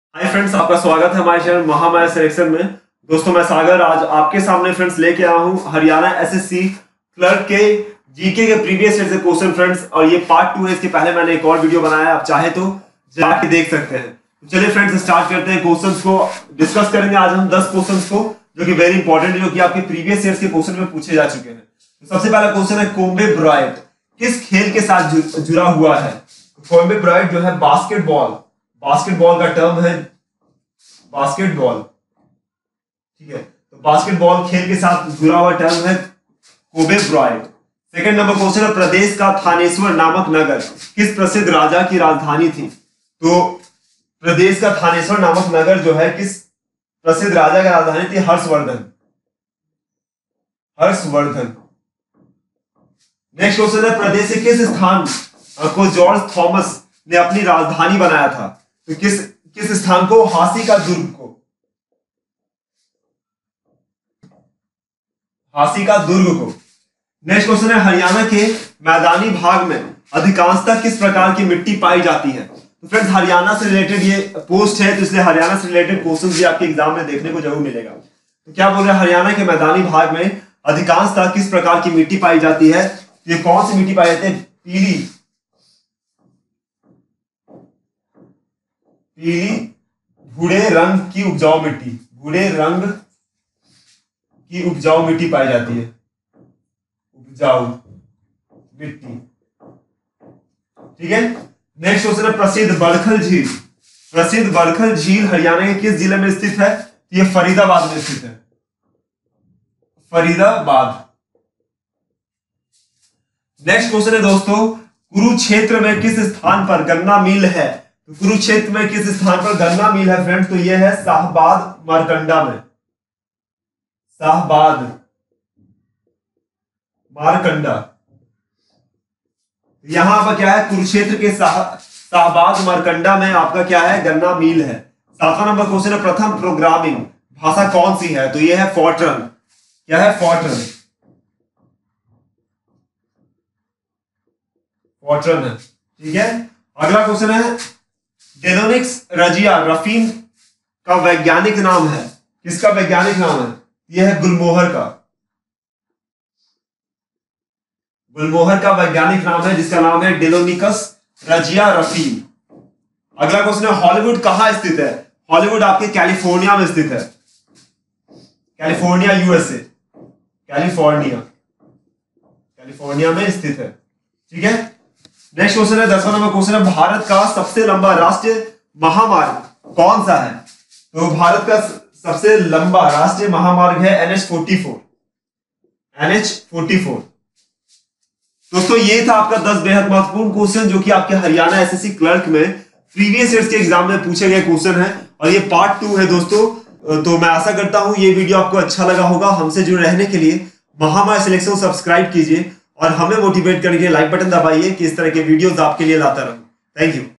HSSC Clerk Previous Question Papers Previous Year G.k Part-2
Learn about HSSC Clerk Previous Question Papers Previous Year G.k Part-2  . These are important question from  Haryana Clerk Exam point of view